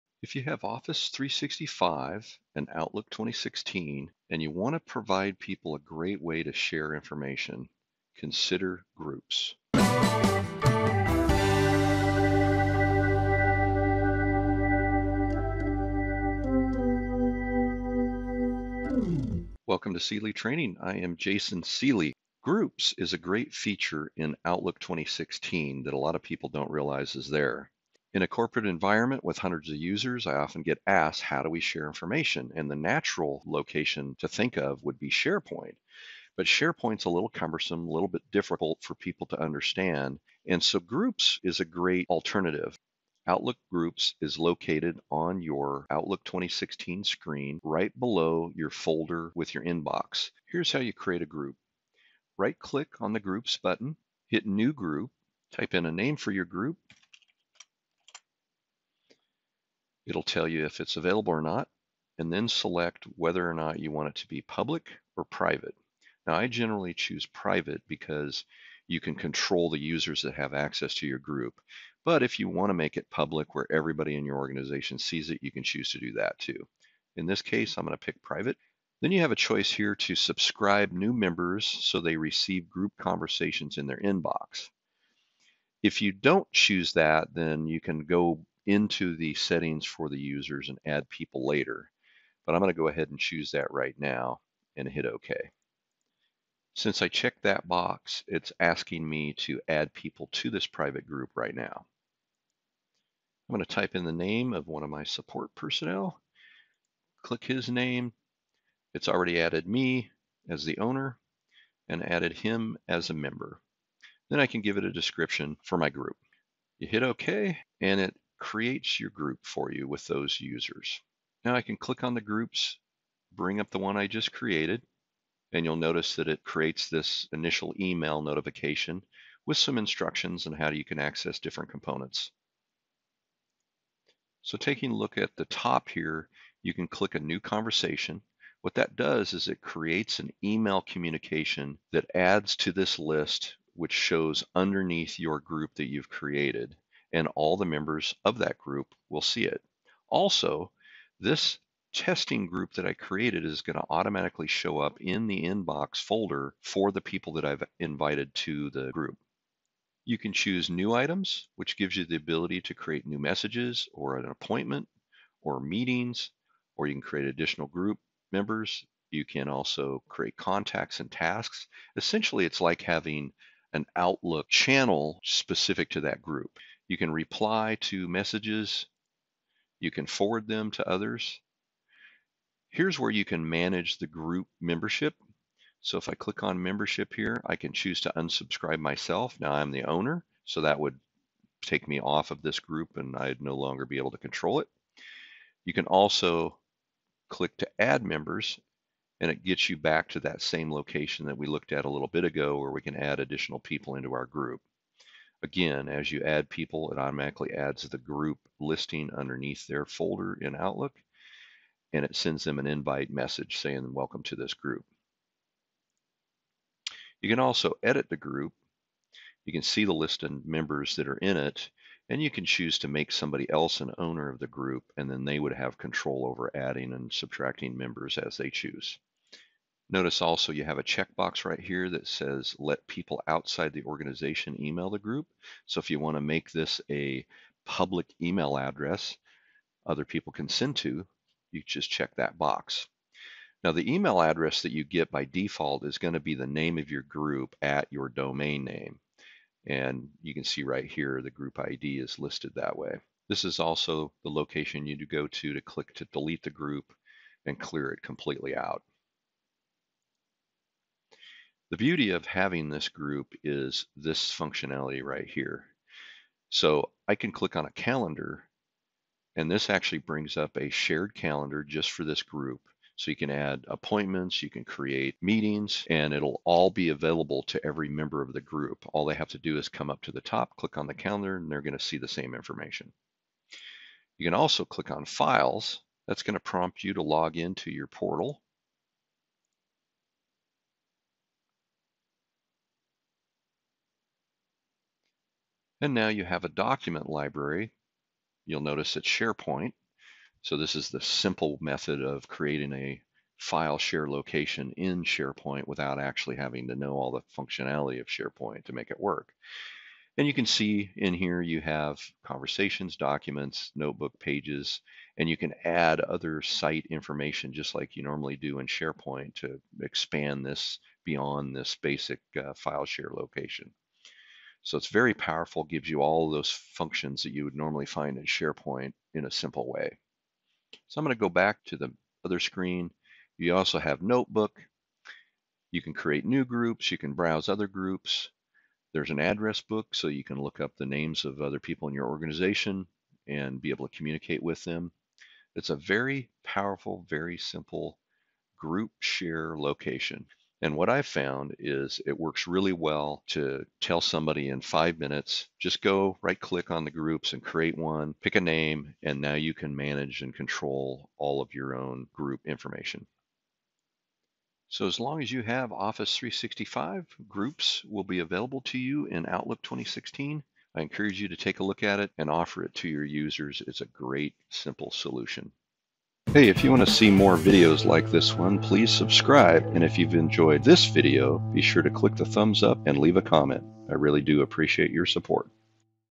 Office 365 Groups
Office 365 Groups is the easiest tool to allow your users to share information without having to build complete SharePoint sites.  Users can create and manage groups all by themselves with very little technical skill.  Create public or private groups to share conversations, calendars, and files within your organization.  Groups will show up under Outlook 2016 at the bottom of your folder list if you have Office 365.